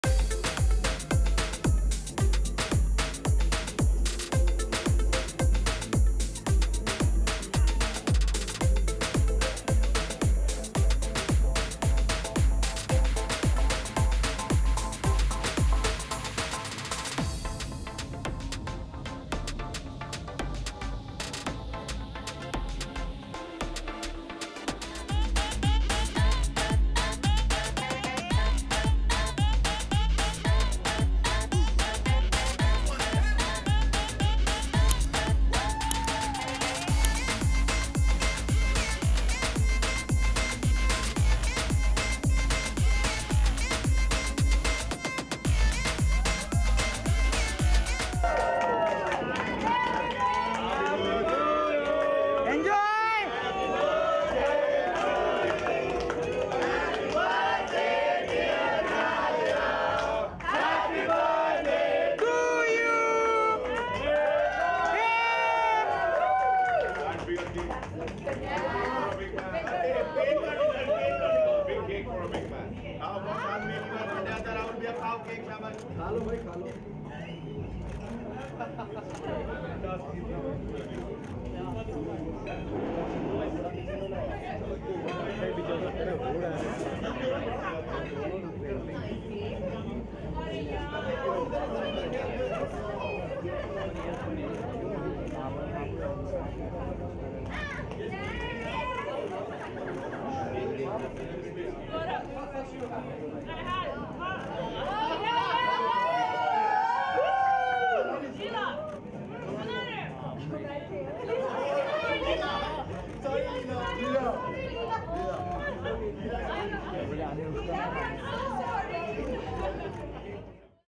Ankita Hugs Neil Bhatt, Aishwarya Greets Vicky Jain | Party Together | Throwback Video
Take a look at the old video where Bigg Boss 17 contestants Neil-Aishwarya & Ankita-Vicky were seen partying together.